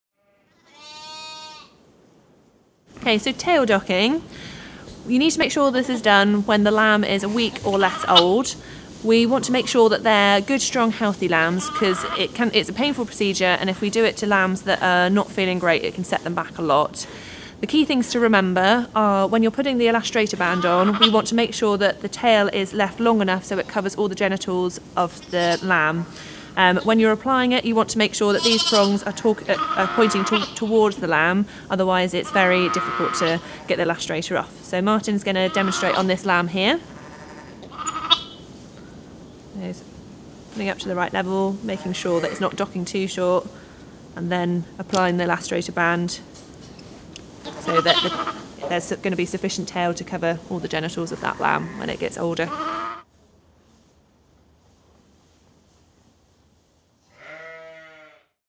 A Tail docking demonstration - Lamlac
Tail docking needs to be done when the lamb is a week or less old and is a key element of caring for baby lambs.

In this video we discuss the key things to remember when carrying out tail docking on your lambs.